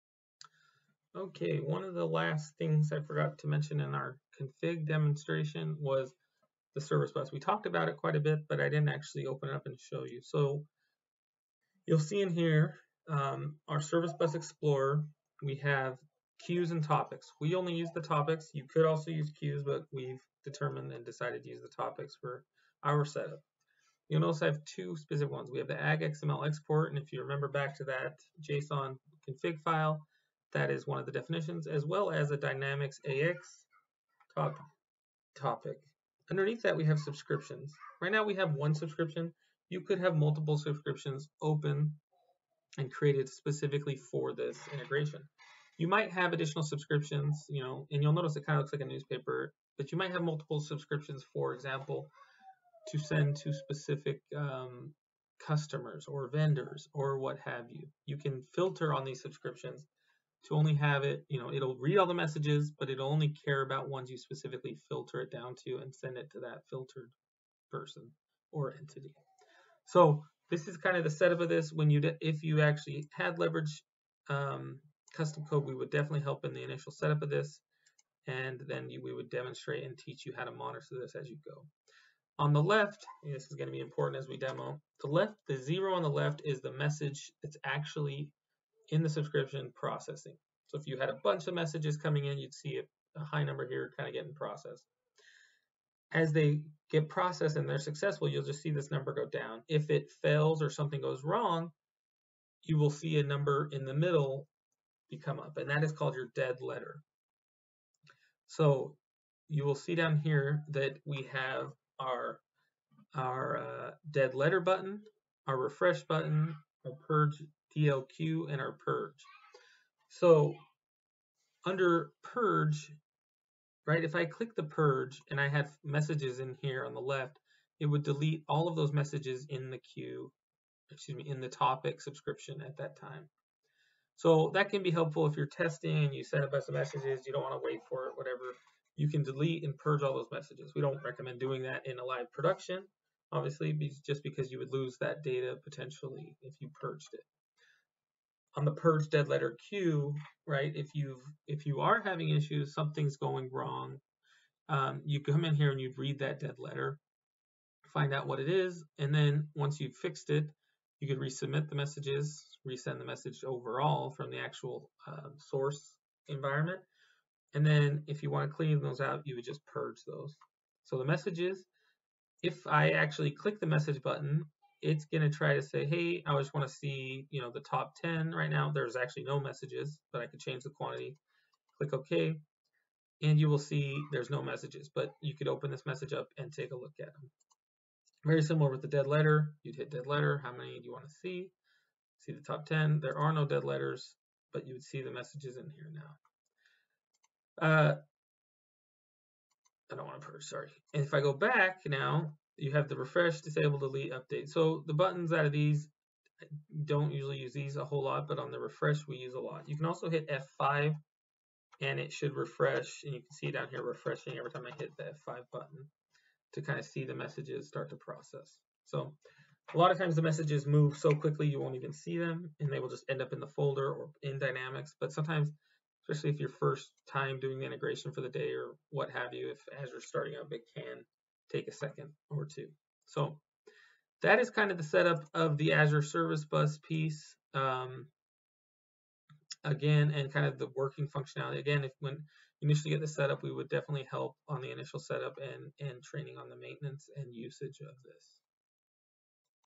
Azure Service Bus Setup (Levridge AgXML Integration #2)
Learn how to set up the Azure Service Bus when creating an AgXML integration in Microsoft Dynamics 365 Finance and Operations with Levridge.

Find out more at Levridge.com